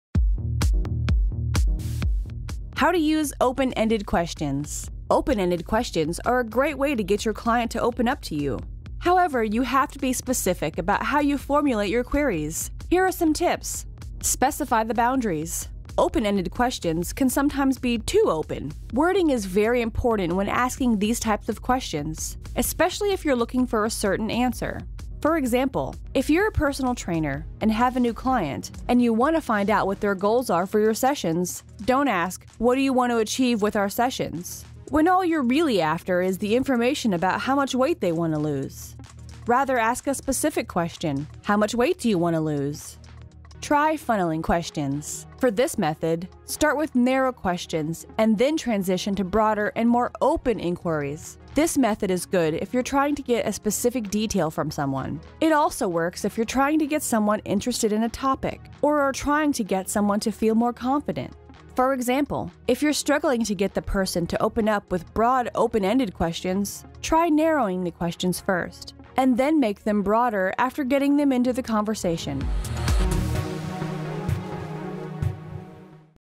How To Use Open-Ended Questions
Open-ended questions are a great way to get your client to open up to you. However, you have to be specific about how you formulate your queries. Here are some tips.

Specify the boundaries. Open-ended questions can be too open sometimes. Wording is very important when asking these types of questions, especially if you are looking for a certain answer. For example:

• If you’re a personal trainer and have a new client, and you want to find out what their goals are for your sessions, don’t ask “What do you want to achieve with our sessions?” when all you’re really after is information about how much weight they want to lose. Rather ask a specific question: “How much weight do you want to lose?”

Try funneling questions. For this method, start with narrow questions and then transition to broader and more open enquiries. This method is good if you are trying to get specific details from someone. It also works if you are trying to get someone interested in a topic, or are trying to get someone to feel more confident. For example:

• If you're struggling to get the person to open up with broad open-ended questions, try narrowing the questions first and then make them broader after getting them into the conversation.